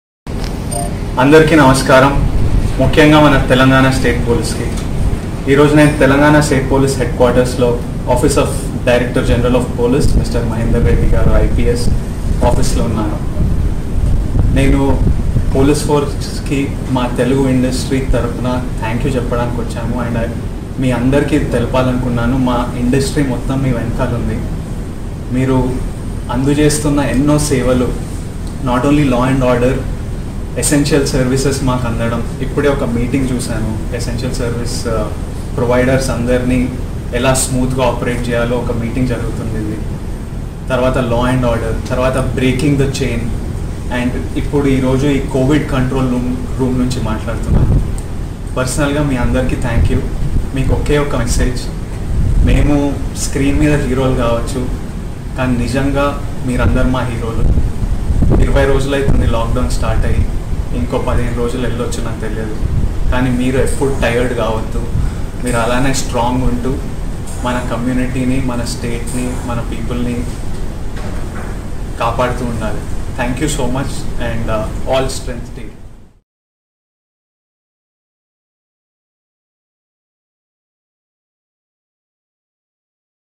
Vijay Devarakonda thanking to Telangana Police
Vijay Deverakonda thanking the police forces for providing services to people during this lockdownindia on behalf of Tollywood